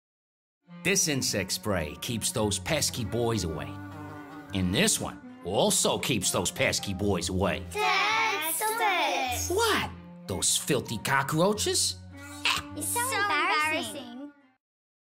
ALDI Cockroaches
Here's a quick one I did a little while back. I had less than 2 days to provide  almost a full minute of single frame, stop-motion footage, as all the girl cockroach characters were shot separately, and composited later. (I only provided the animation, not the puppets or sets for this one.)